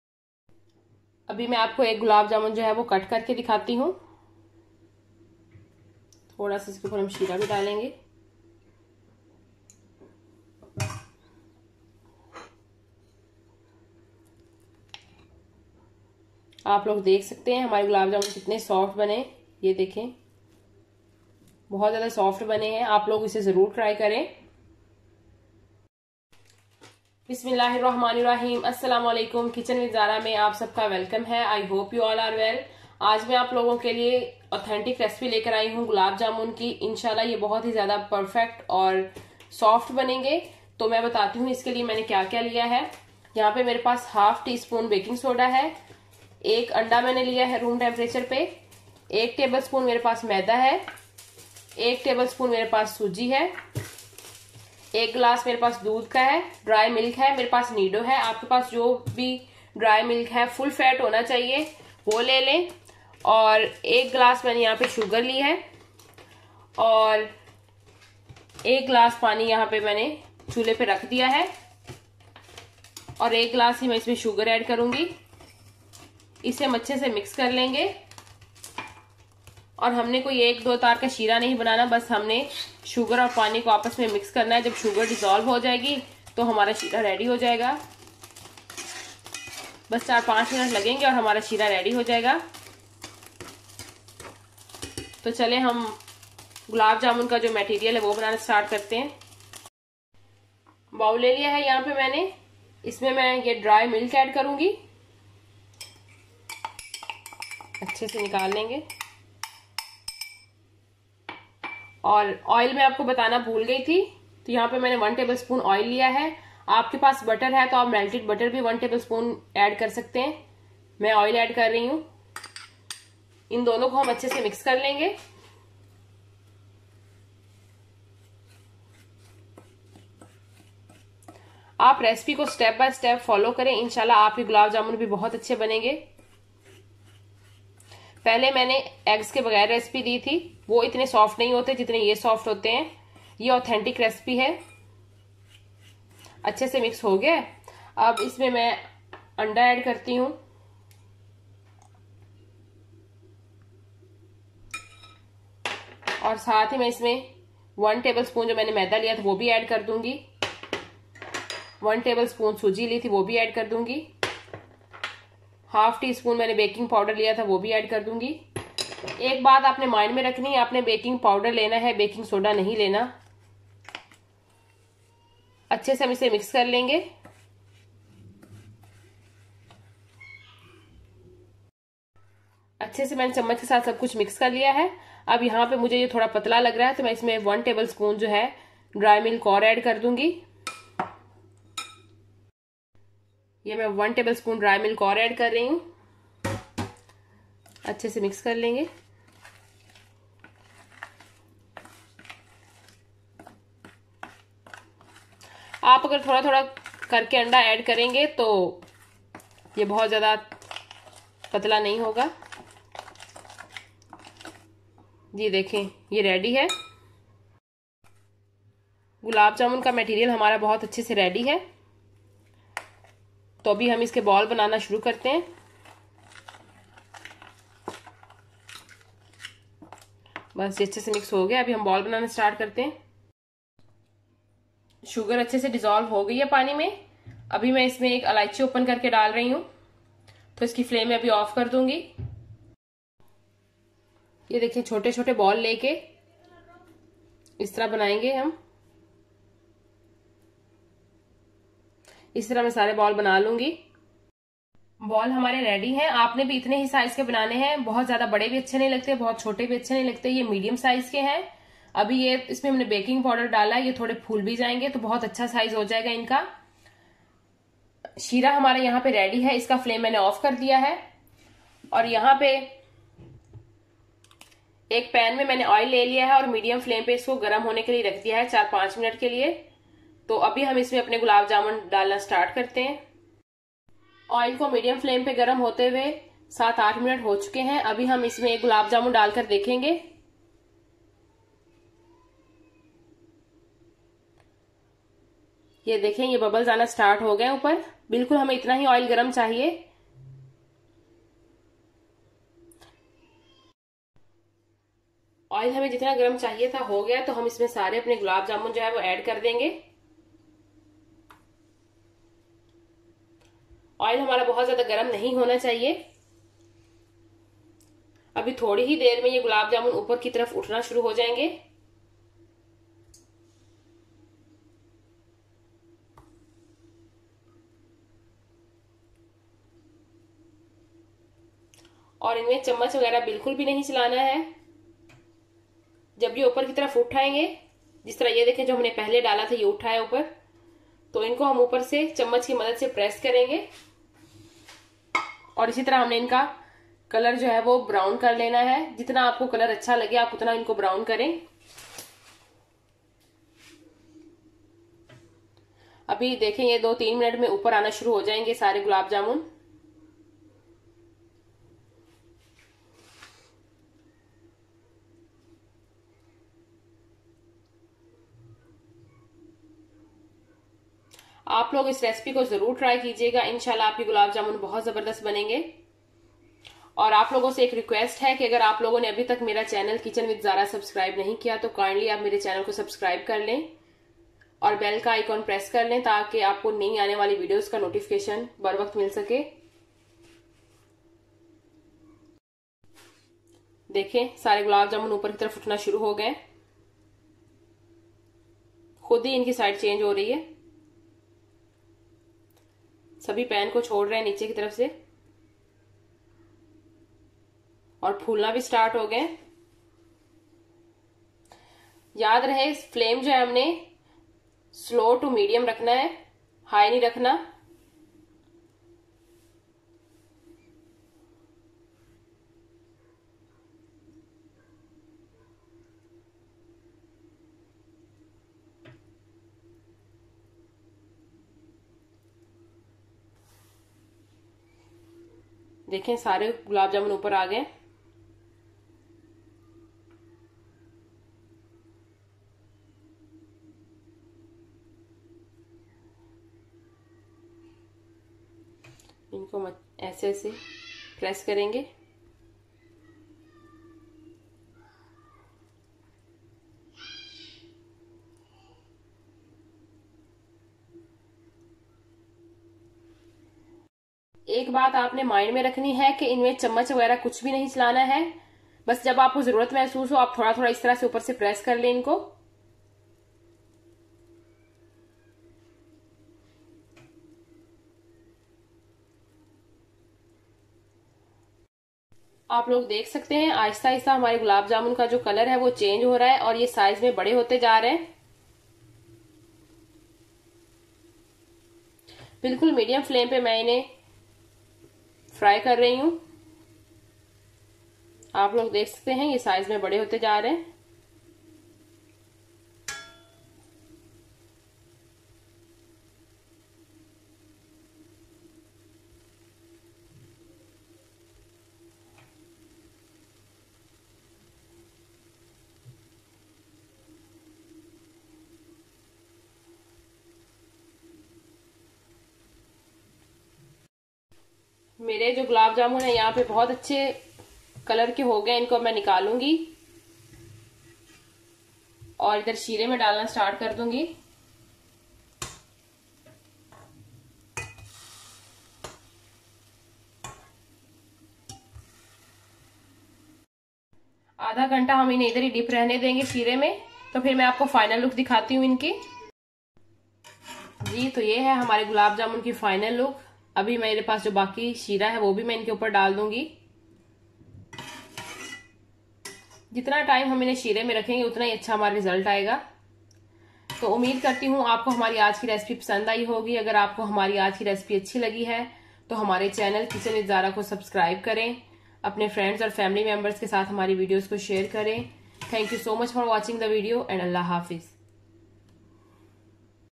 Gulab jamun recipe with eggs || soft and juicy gulab jamun recipe
Gulab jamun recipe with eggs

Written recipe in English ;
Ingredients for sugar serum :
Green cardemon 1

Directions for sugar serum;
In a pan put all the  ingredients and let the sugar desolve in the water and turn off the flame. Sugar serum is ready.

Ingredients for gulab jamun material ;

1 cup milk powder
1 tbsp of all purpus flour
1 tbsp of semolina
1 tbsp of butter / oil
1/2 tsp of baking powder

Directions :
Mix all the ingredients in a bowl and then make round balls and in a pan take some oil to fry the gulab jamun. Oil should be heated on very low flam and add the gulab jamun and fry them on very low flame until the colour of gulab jamun changes to brown. Now take out the gulab jamun and dip them in sugar serum  gulab jamun are ready to eat for good results aip them in sugar for 3 hours at least kindly subscribe kitchen with Zara.